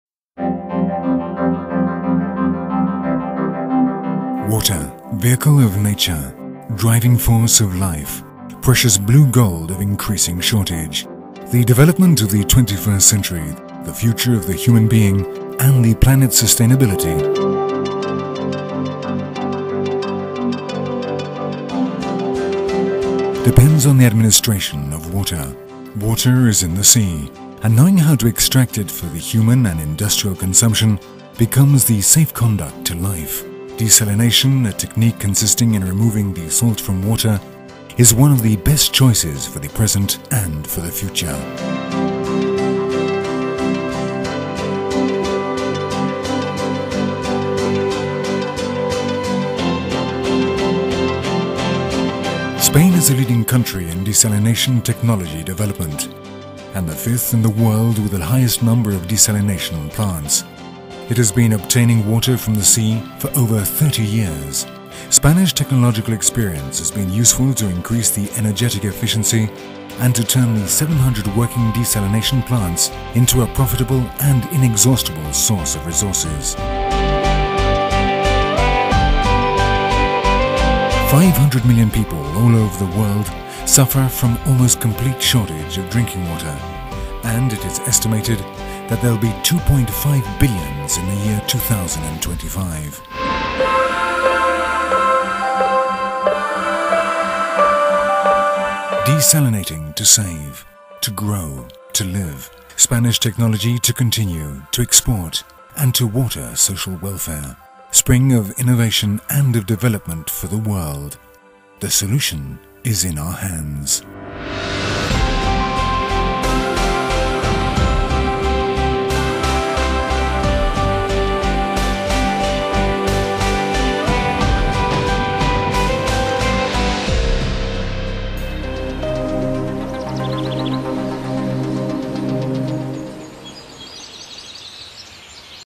Desalination in Spain - ICEX
Spain is a leading country in desalination technology development and the fifth in the world with the highest number of desalination plants.